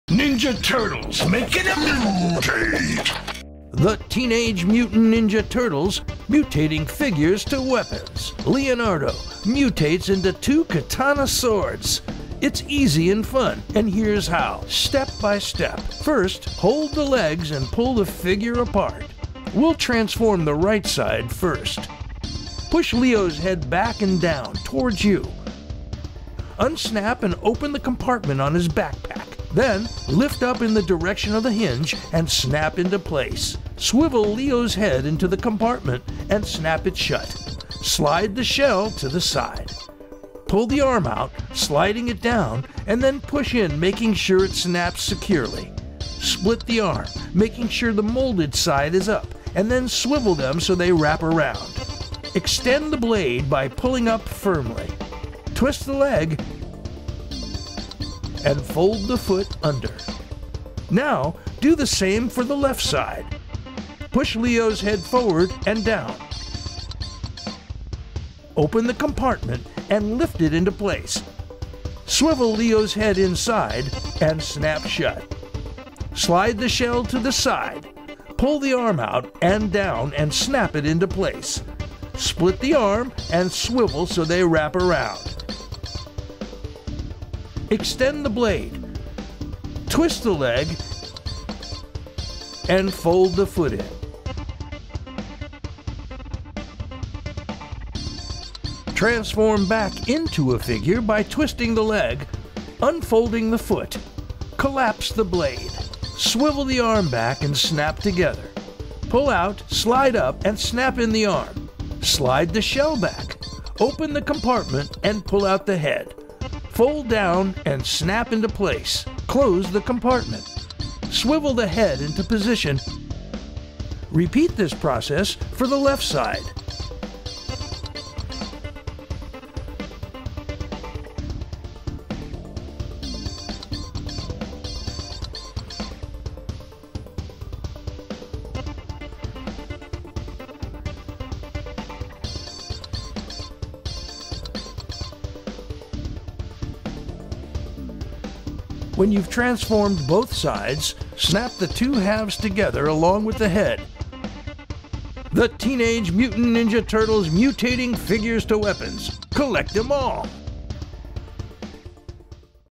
TMNT Mutations Leonardo to Sword Instructional
Mutated from ooze and raised in the ways of the Ninjutsu by their rat sensei, Splinter, the Teenage Mutant Ninja Turtles are ready to venture outside their sewer lair to fight evil everywhere! The Ninja Turtles transform into their powerful iconic weapons to fight the forces of evil!